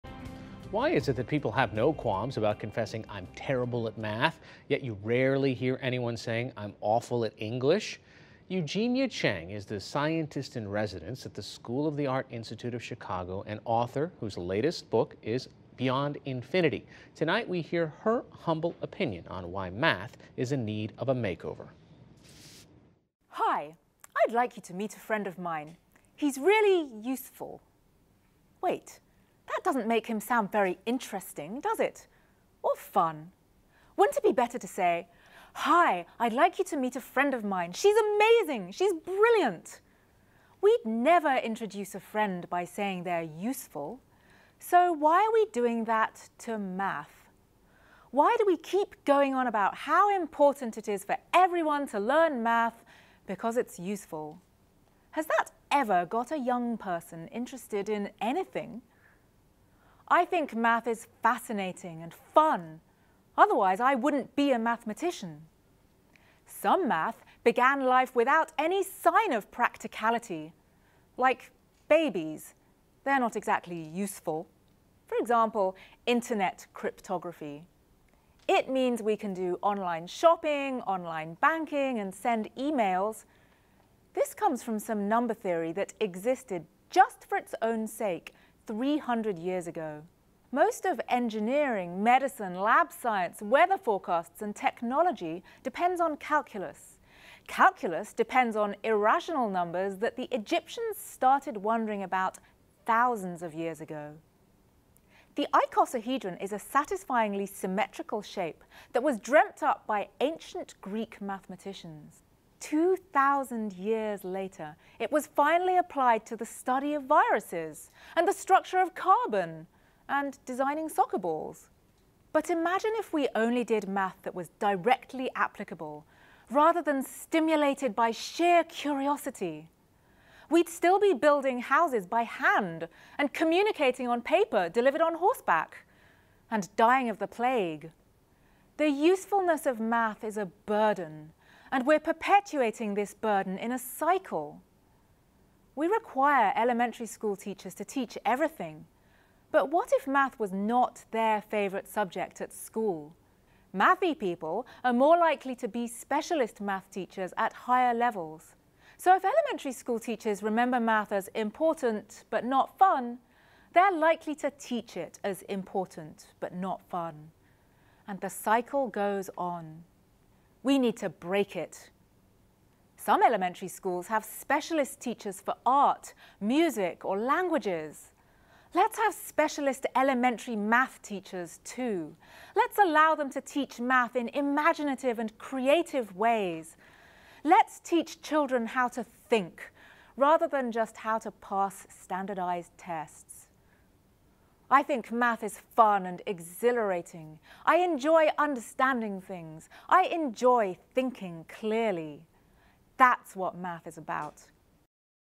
Math is amazing and we have to start treating it that way
Numbers and algorithms aren’t just useful to mathematician Eugenia Cheng -- they’re exhilarating. That’s why she’s on a mission to help transform math’s notoriously boring reputation in education into one of thrilling wonder and creativity. Cheng gives her humble opinion on how to make math really matter to students.